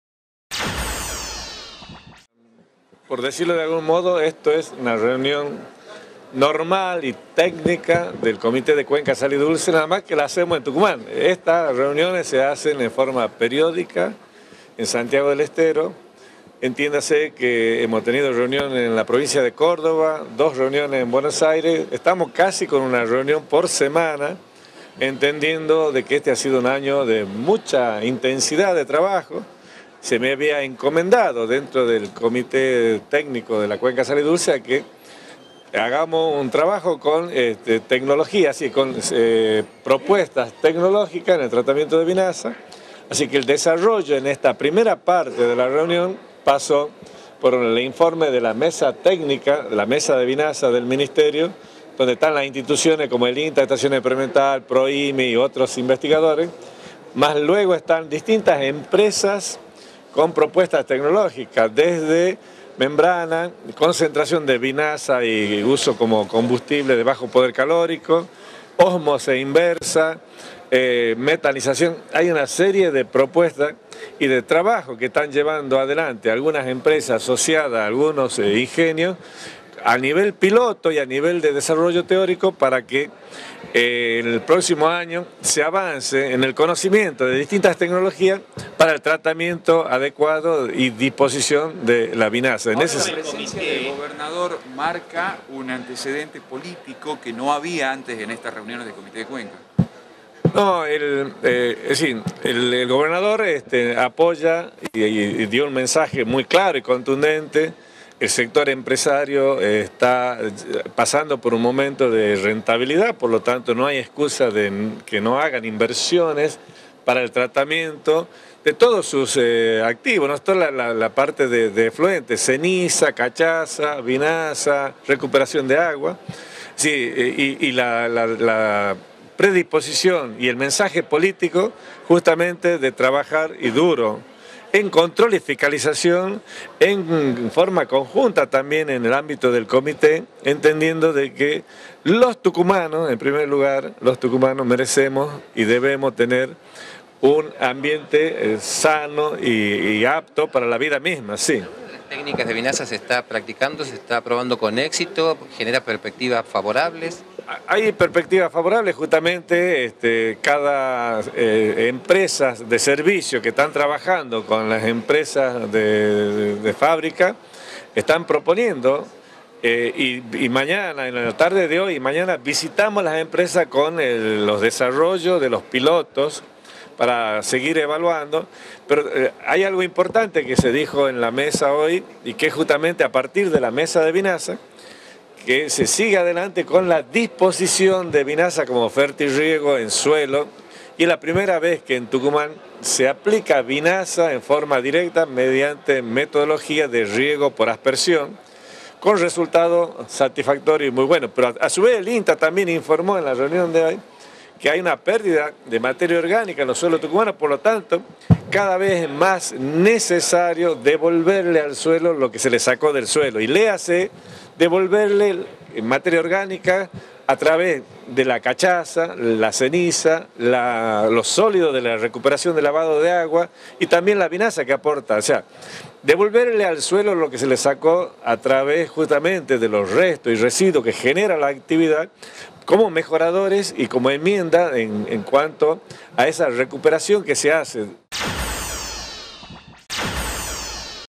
Nuevas tecnologías para el tratamiento de la vinaza
nuevas tecnología para contaminar menos los causes delos rios